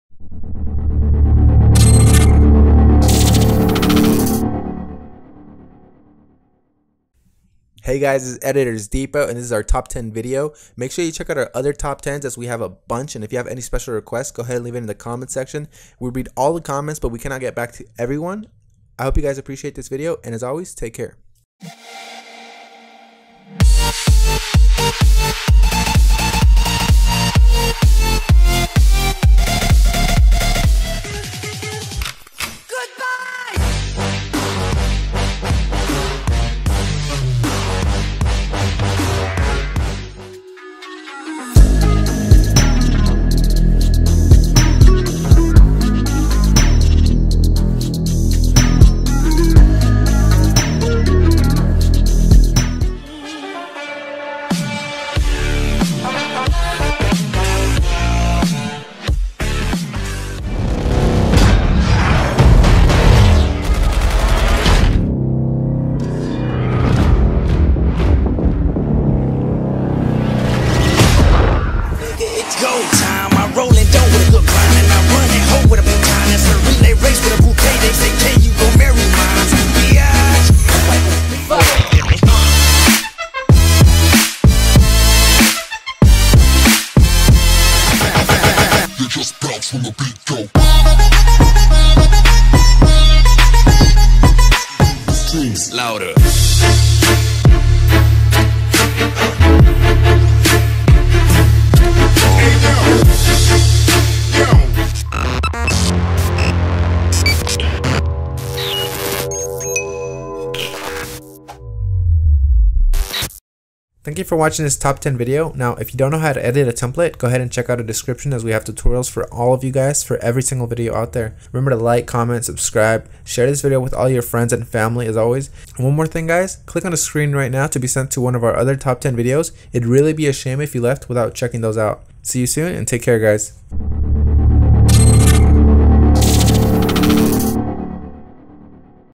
Top 10 Sony Vegas Intro Templates #4 - FREE DOWNLOADS
Sony Vegas templates certainly aren't common, but we have some amazing templates included in this top 10! These templates include clear colors, shockwaves and much much more. All of these templates are free to download!